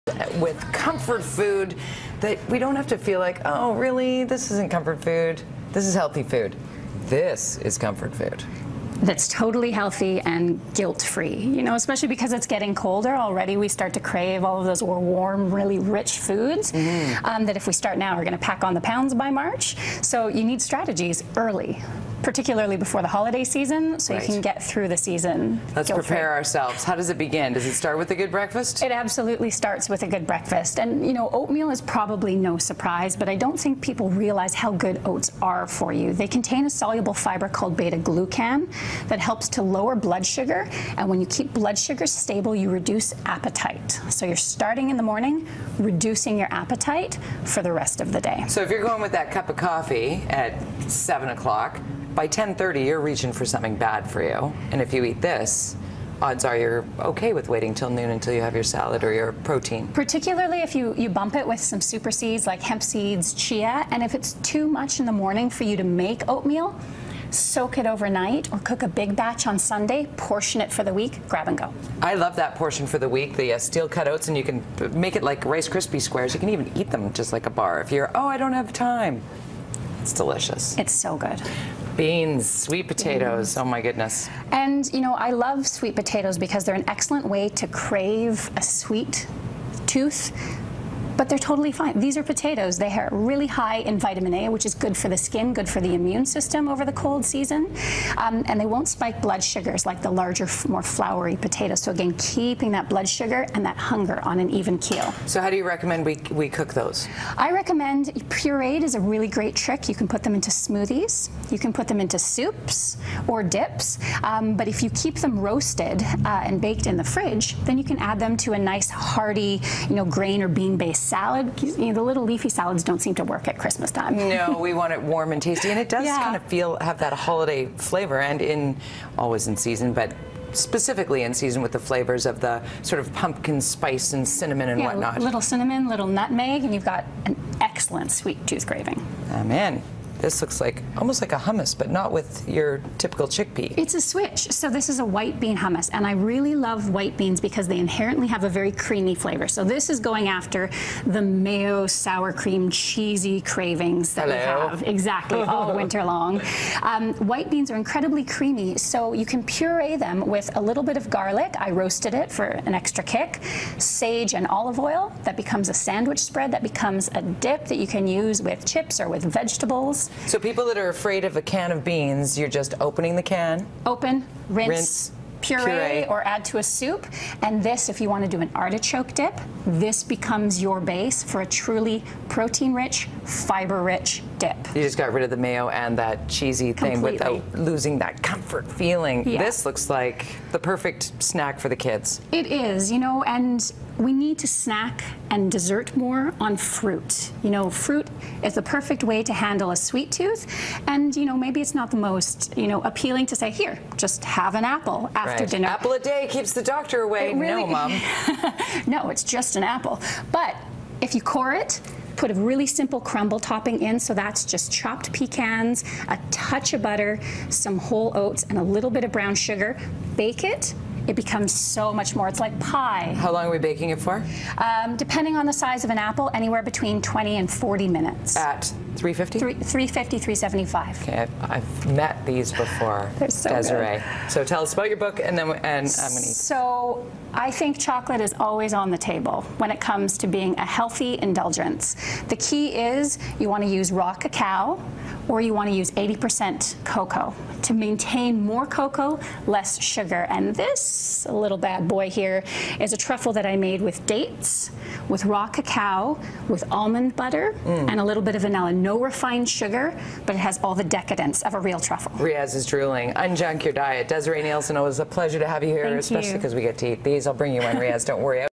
Healthy Comfort Foods
Maybe it’s nostalgia, or maybe it’s the carbs – we all have our favourite comfort foods. Registered Dietician Desiree Nielsen.